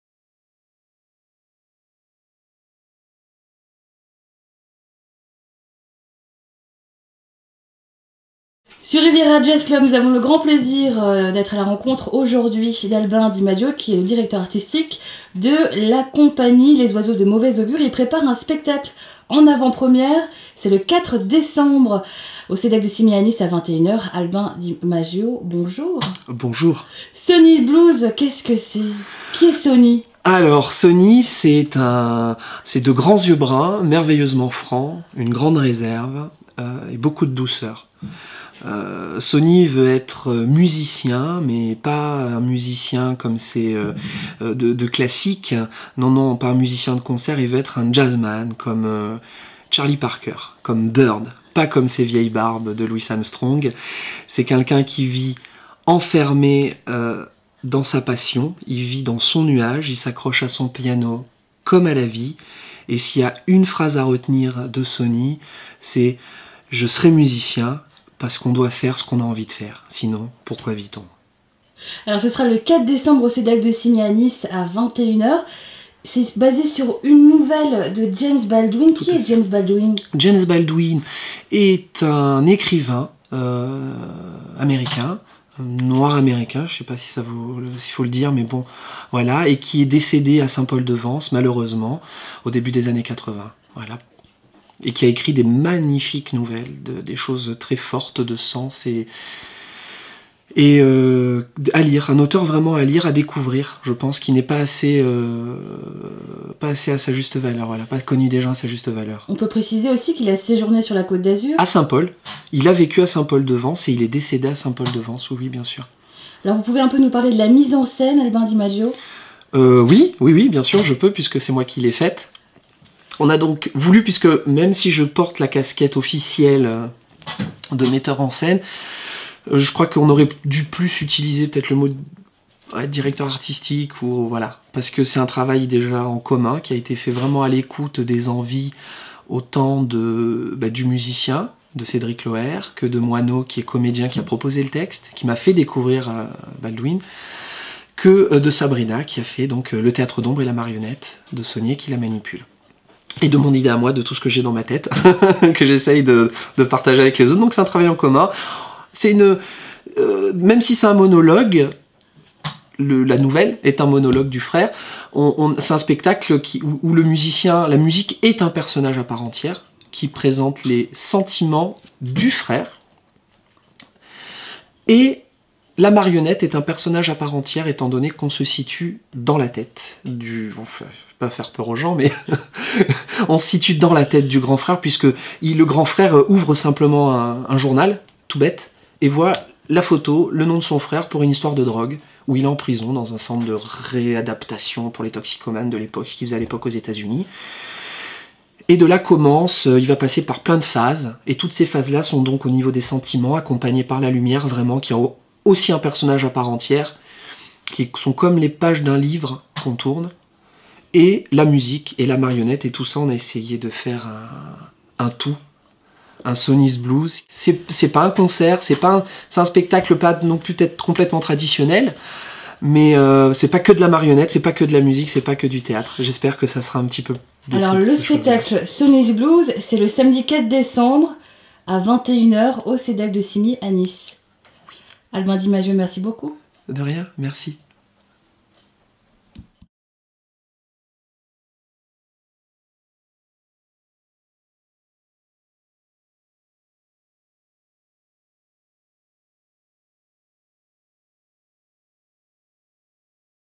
Sonny's Blues albin Di MAggio
Au CEDAC de CIMIEZ

Théâtre Jazz-Marionnette- D'après une nouvelle de James Baldwin

Mise en scène Albin DI MAGGIO

Avec

Comédien:Moineau, Marionnettiste:Sabrina Anastasio, Musicien:Cédric Lauer

Photographe : Eric Ivaldi

 WHO'S SONNY ?

De grands yeux bruns merveilleusement francs,

une grande réserve et beaucoup de douceur.

Appréhendé au cours d'une descente de police

dans un appartement du centre de la ville, pour avoir

vendu et pris de l'héroïne.

« Je serai musicien...Ce que je veux c'est jouer avec

des musiciens de jazz. Je veux jouer du jazz. »

Il vivait enfermé dans un nuage, dans une passion,

une vision personnelle. Il n'y avait aucun moyen de l'atteindre.

Sonny s'accrochait à ce piano comme à la vie.

« Je crois qu'on doit faire ce dont on a envie. Sinon, pourquoi vit-on ? »

21h EN AVANT PREMIERE

Suivi d'un cocktail

19h EXPO PHOTO « Série Urbaine » d'Eric Ivaldi

Suivi d'un apéritif dinatoire

TARIF UNIQUE: 16 €*

*Une partie des entrées sera reversée au Téléthon 2010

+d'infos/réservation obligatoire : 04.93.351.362